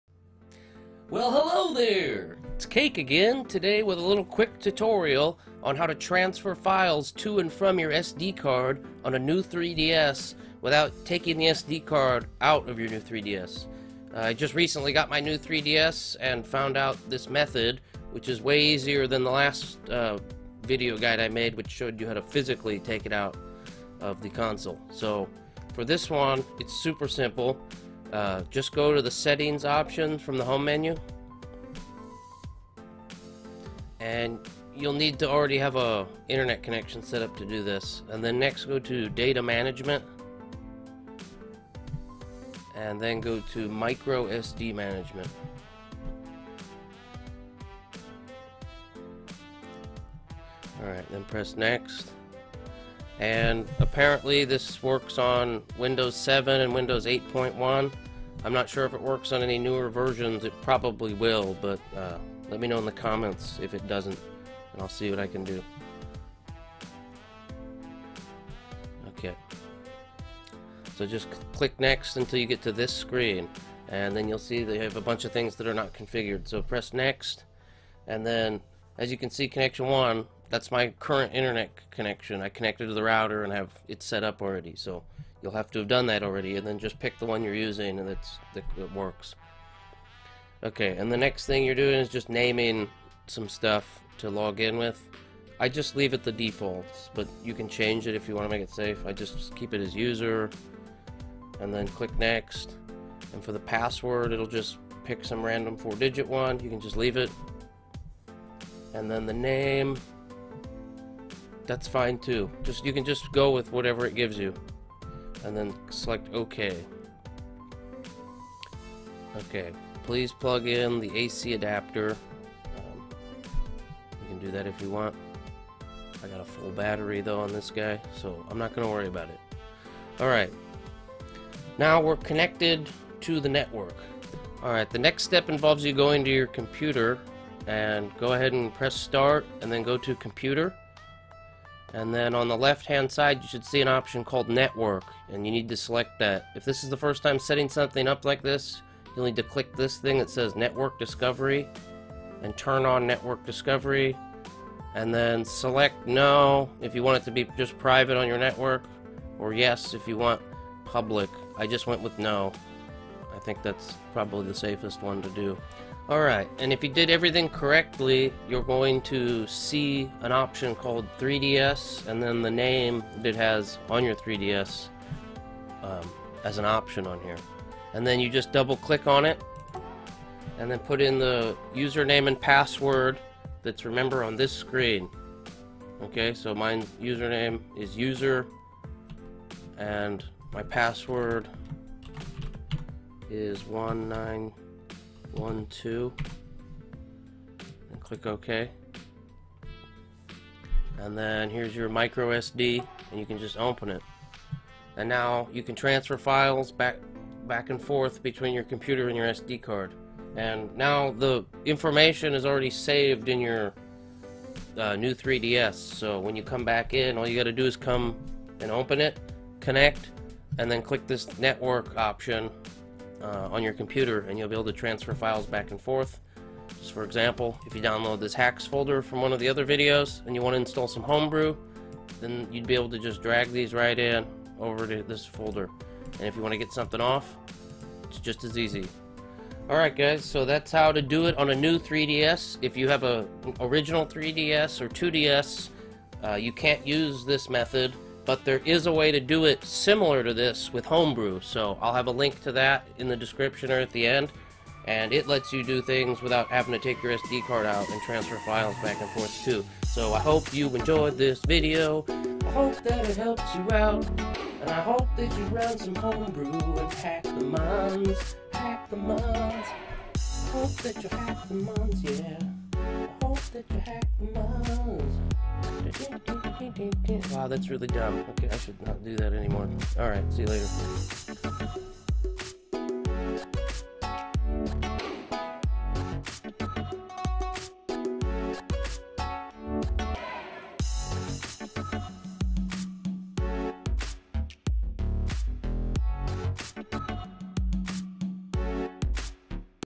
Transfer Files to a New 3DS & XL with Data Management
How to transfer files to and from your computer and New 3DS with Data Management. Only works on New 3DS and New 3DS XL